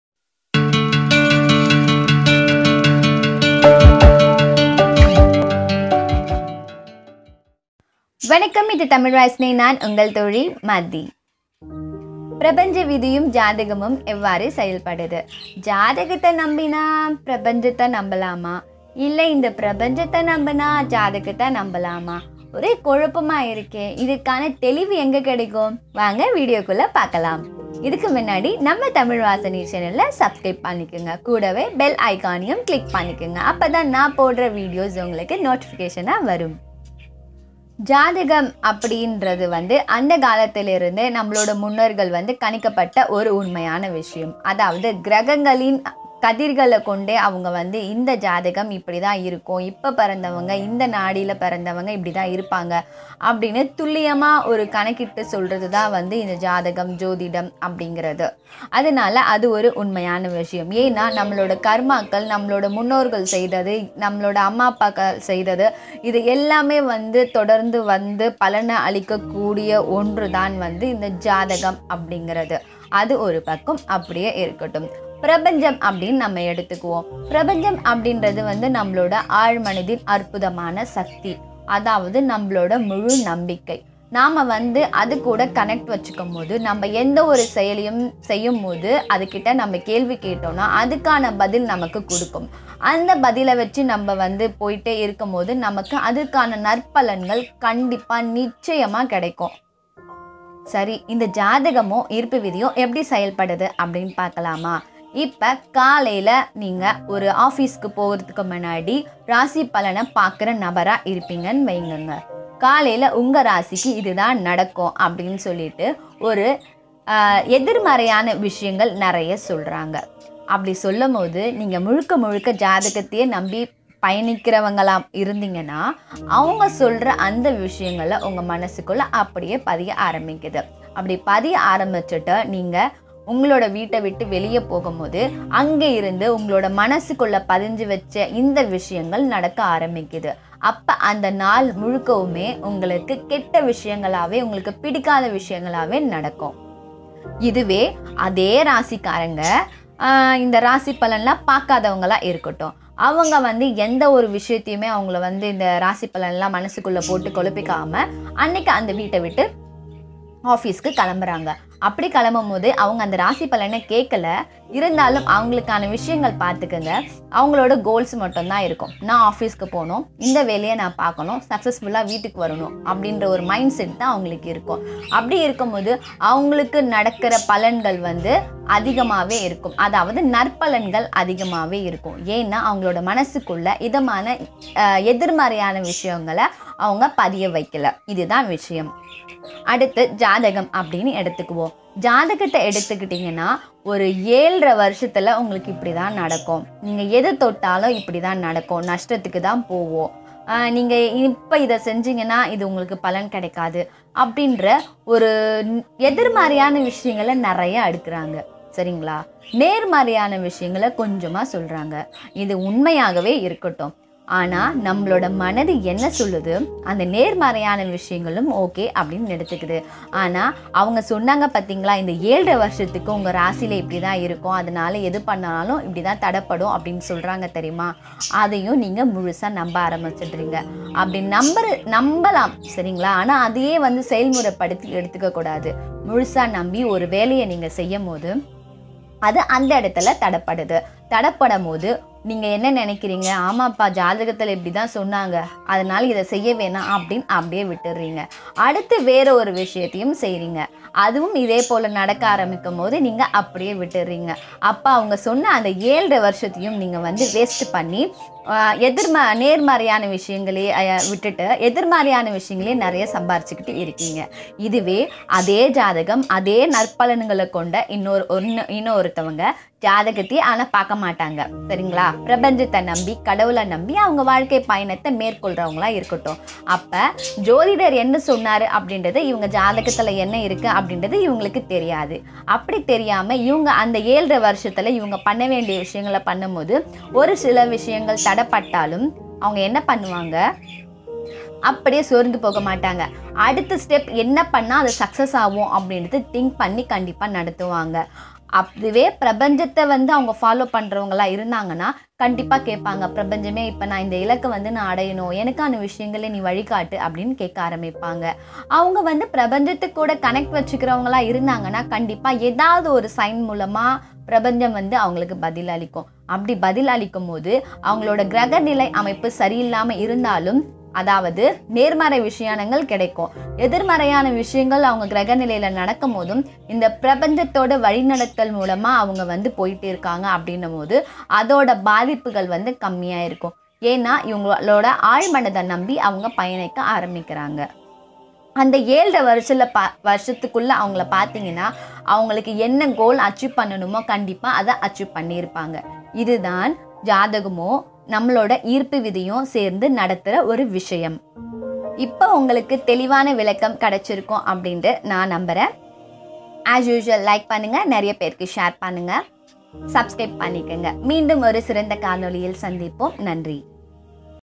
பிரபஞ்சமும் ஜோதிடமும் எவ்வாறு செயல்படுகிறது/The Universe and Horoscope in tamil
This video gives info about law of attraction in Tamil

தமிழ் வாசனை

      ஜாதகம் ஜோதிடம் சொல்வதை நம்பி இருக்கலாமா?அல்லது பிரபஞ்ச சக்தியை நம்பி செயல் படலாமா?ஒரே குழப்பமா இருக்கா தீர்வு இந்த வீடியோவில் உள்ளது.

link

-Mathi